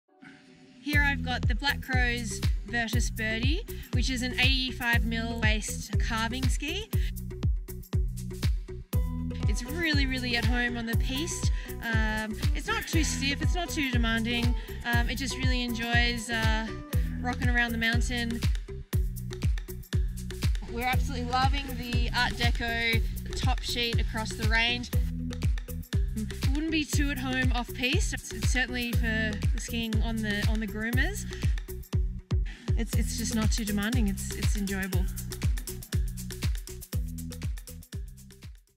Black Crows Vertis Birdie 2020 Ski Review
aussieskier.com review the 2020 Black Crows Vertis Birdie Womens Skis in Pila Italy.

Updating the Ova Birdie, the Vertis Birdie is an awesome All Mountain Ski for those looking to stick on or close to the groomers. The short radius combined with narrow waist make for a delightfully nimble ski that can be pushed hard on-piste.